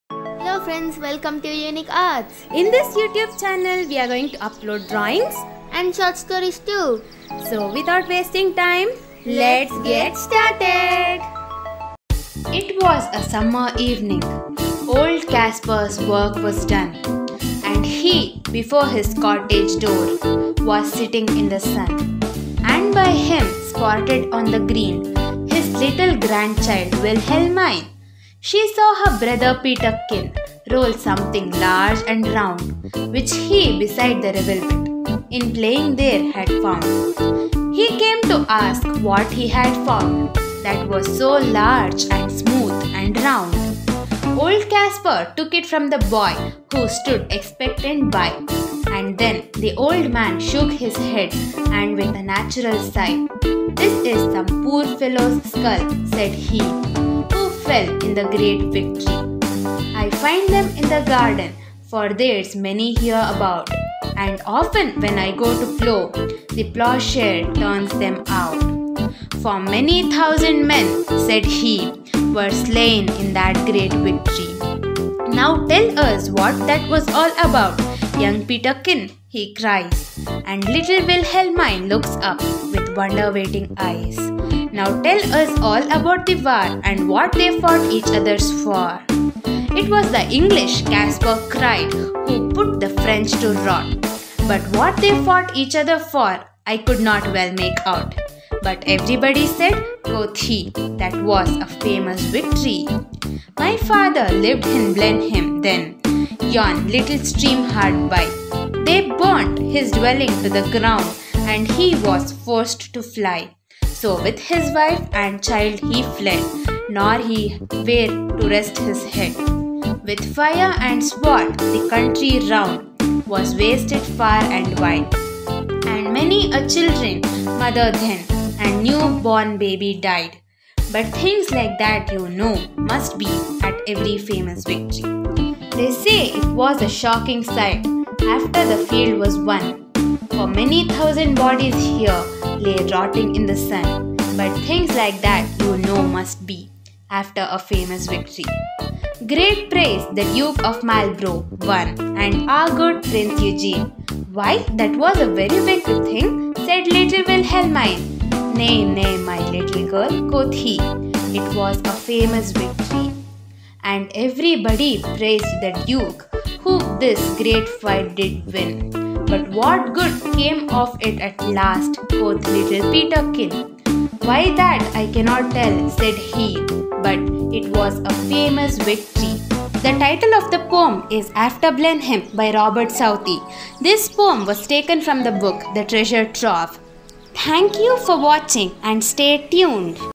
easy mandala art for beginners/mandala patterns/After Blenheim poem recitation/The Uniq Art
Hello friends in this video we have uploaded a mandala pattern along with After Blenheim poem recitation.